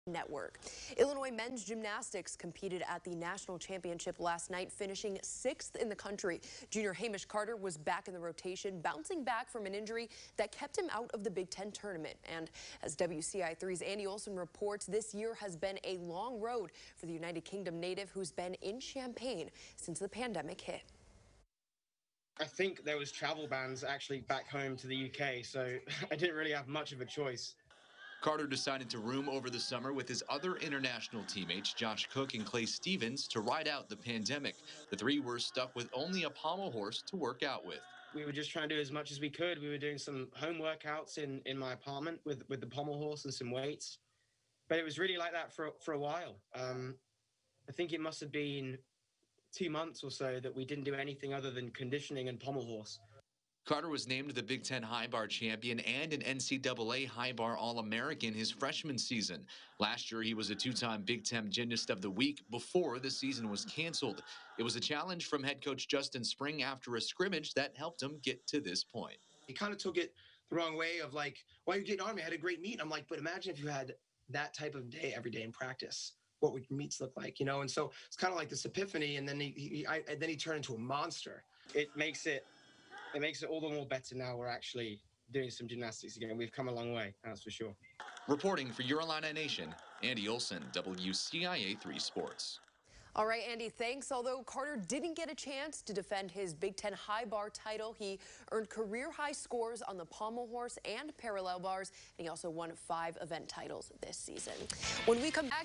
Hamish Carter competes at NCAA's after missing Big Ten Championship
CHAMPAIGN (WCIA) -- Illinois Men's Gymnastics competed at the National Championship last weekend, finishing sixth in the country. Junior Hamish Carter was back in the rotation, bouncing back from an injury that kept him out of the Big Ten Tournament. It's been a long road for the United Kingdom native who wasn't able to go home this summer, and has been in Champaign since the pandemic hit.